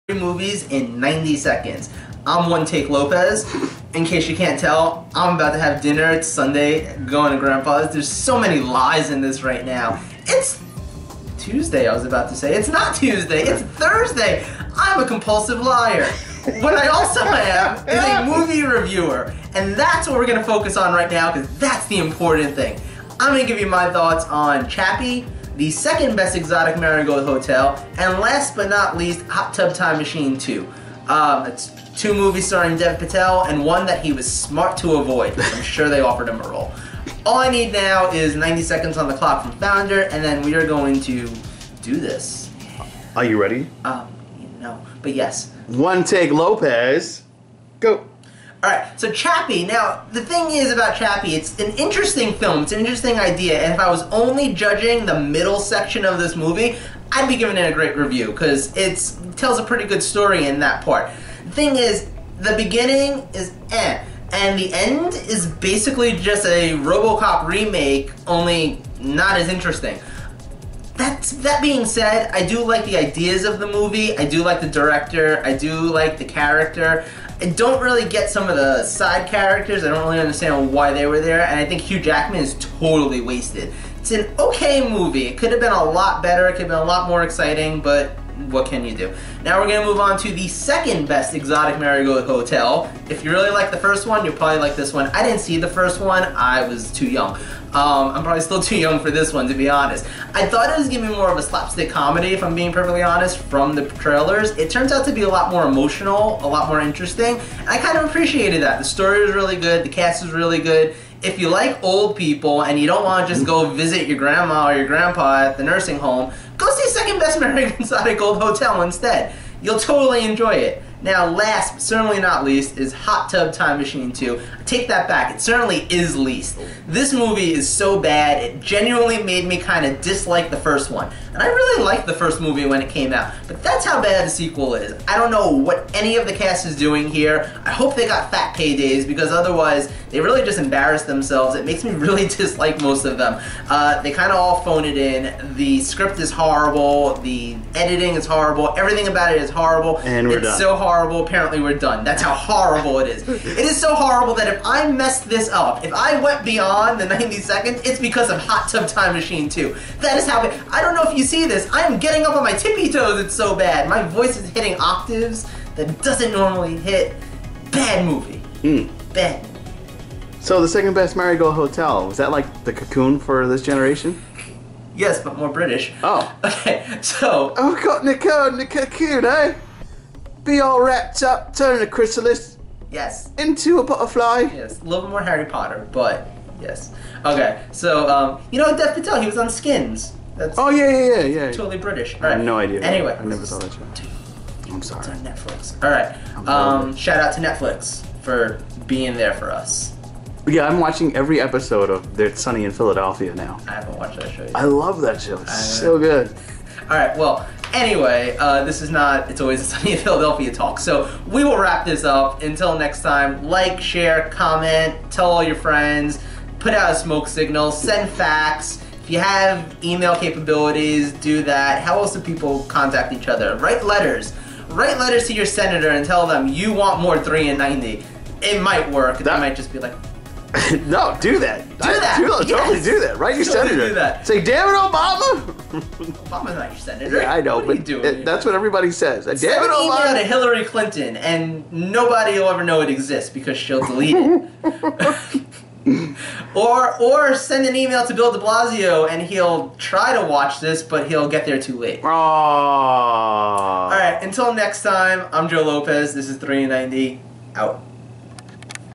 2nd Best Exotic hotel, Chappie, Hot Tub Time 2, 3in90
3in90 back with Hottub Time Machine 2, 2nd Best Exotic Hotel, and Chappie.

Please watch our new weekly venture into the dark harddrive for music old and new called "Disk Dungeon with Joe Dove &  Julian "